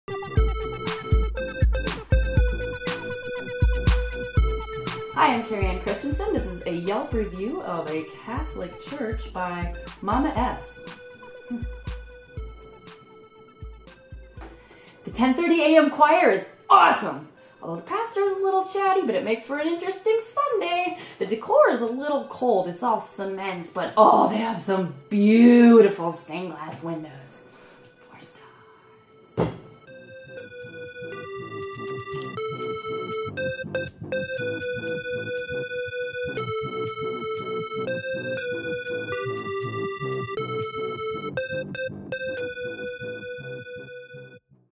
Yelp Monologues 01 - Catholic Church by Mama S.
Business: San Bruno Catholic Church
Yelper: Mama S.
Location: San Bruno, CA